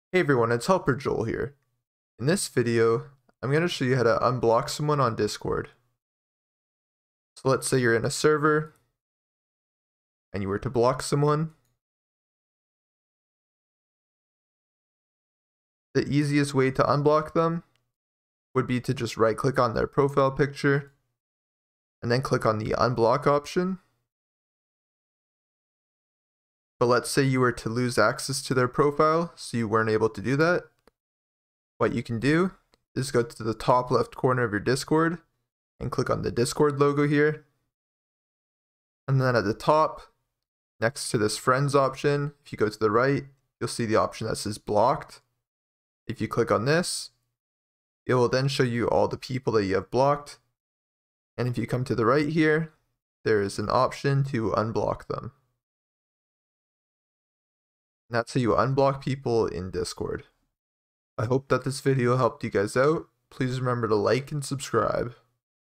How To Unblock Someone On Discord (PC)
In this video I show you how to unblock someone on Discord. Unblocking someone in Discord is quick and easy to do.